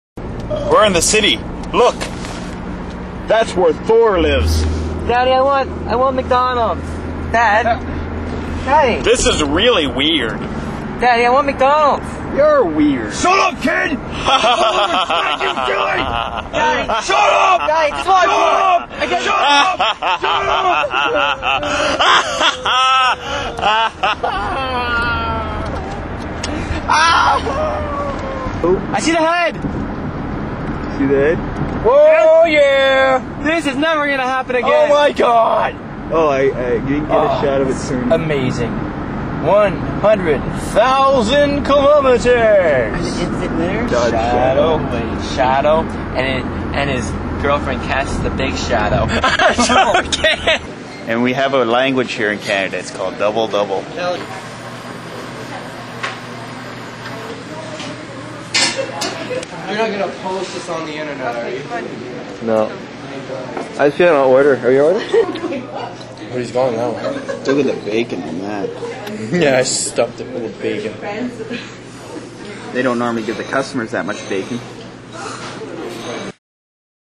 Having fun with a Video Camera 2
Carleton Place Boys in ottawa
just a bunch of clip i put together one sunday afternoon when i was board after we shot it